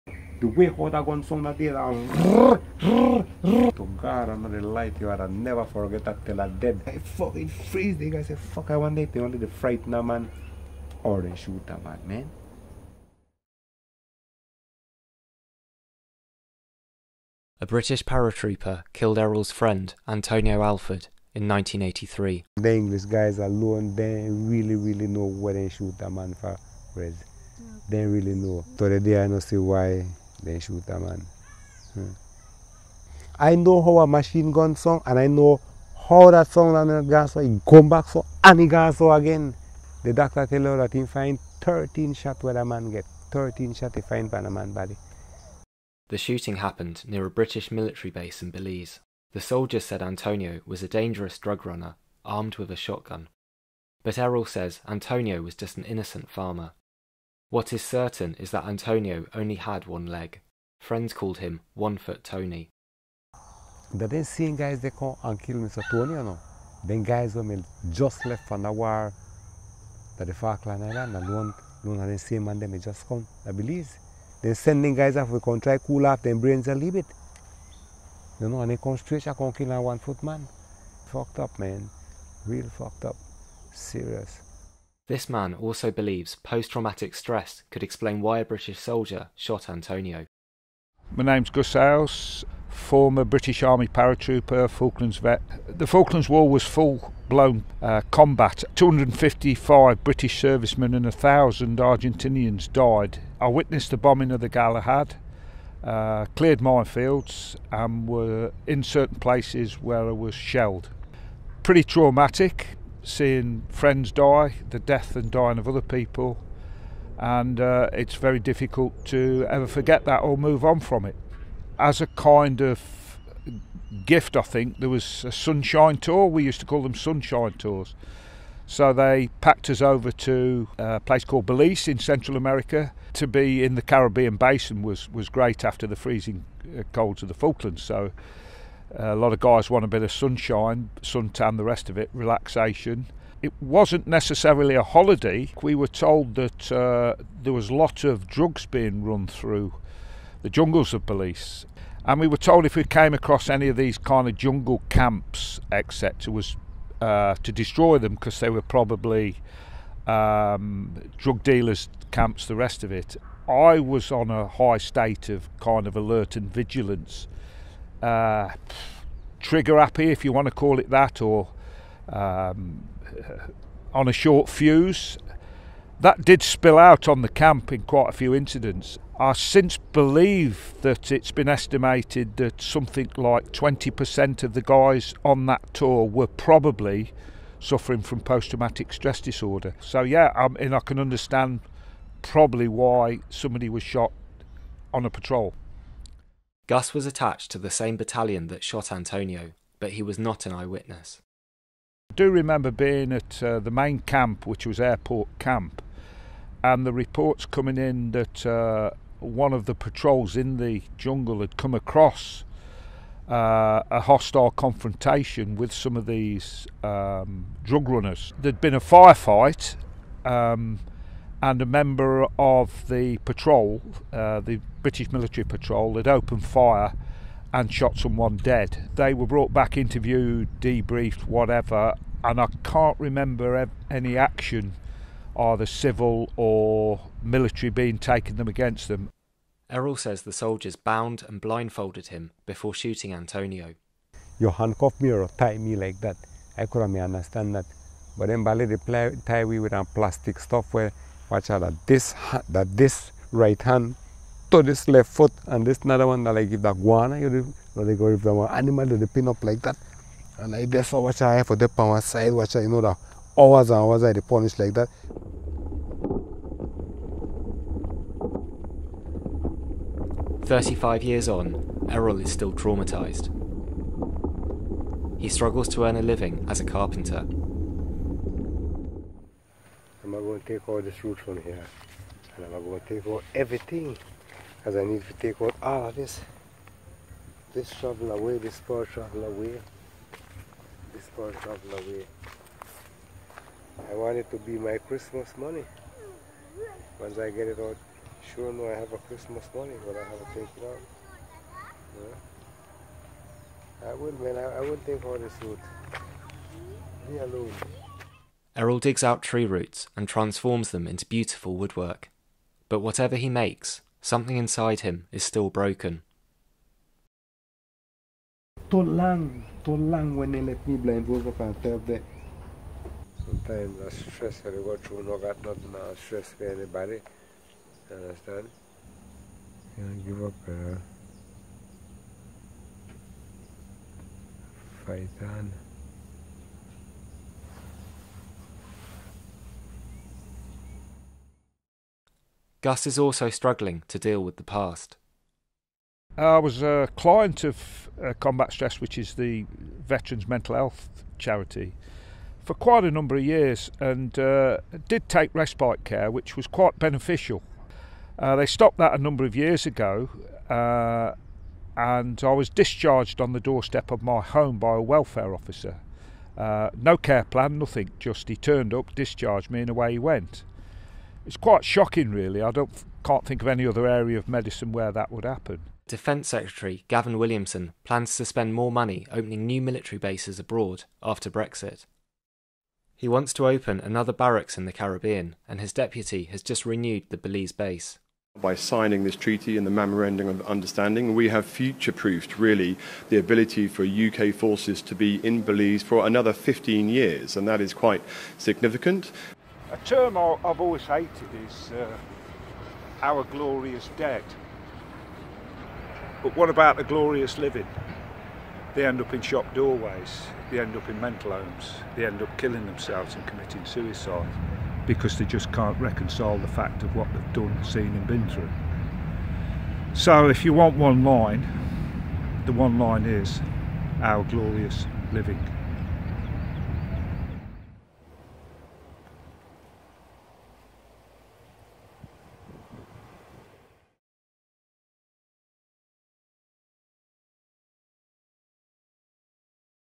One foot down – the strange shooting of Antonio Alford
In 1983, a British paratrooper shot dead 55-year-old Antonio Alford in Belize, near a UK military base. The British army said Antonio was a cannabis grower who pointed a shotgun at the patrol commander. But local people say Antonio only had one leg, after a shark accident. Known as 'One foot Tony', he was a friend of Belize's prime minister and helped run his local football club. So did he really pose a lethal threat? 35 years later, Phil Miller traveled to Belize and tracked down an eye-witness to this controversial killing, Errol Flowers. He is still traumatised by what happened and is accusing the British army of murdering Antonio.

Produced by Phil Miller for New Internationalist